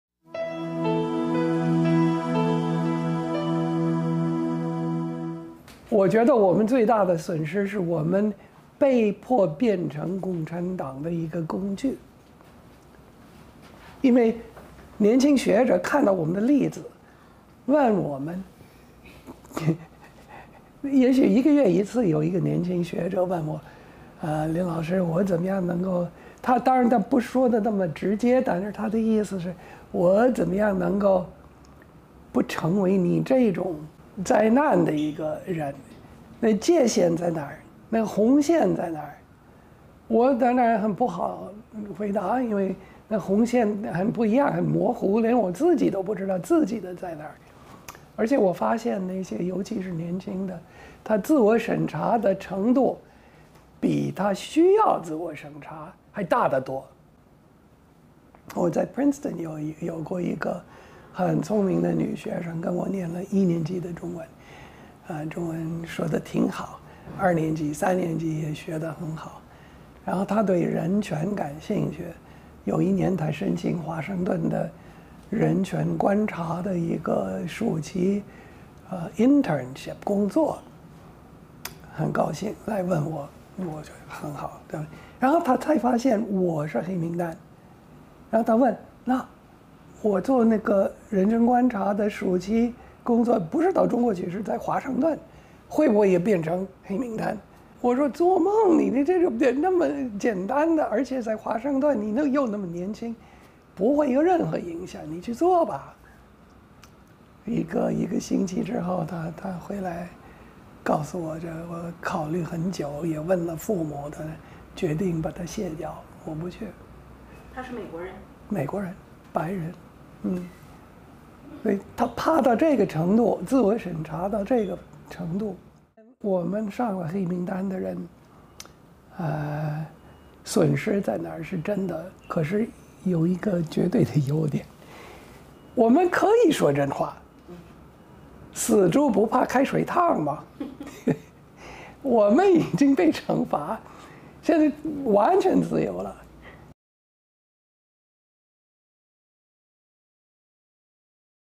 遭列黑名單 林培瑞:有失有得｜中央社專題報導
在中共的歷史遺忘術下,林培瑞(Perry Link)認為最可惜的是年輕人,他們在學校學不到真實的六四,家人也基於保護不願意多談;但他認為,有一天當年輕人接觸到六四真相,還是有可能相信的。